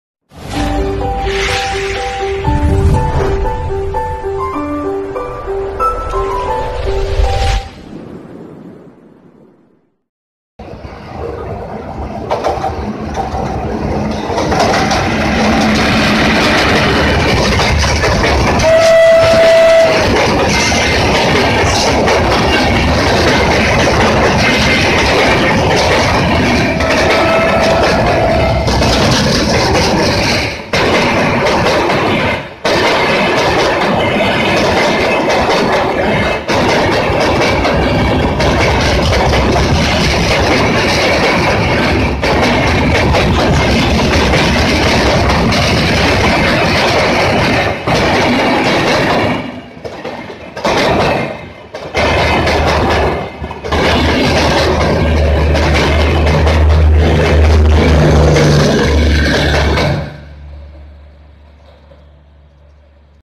150th SUBSCRIBER SPECIAL:Cracking sound of track by Mahabodhi express near Anand Vihar
12397 Gaya New delhi Mahabodhi express passing through anand vihar and gives cracking sound of track and in awesome speed
Location: Anand Vihar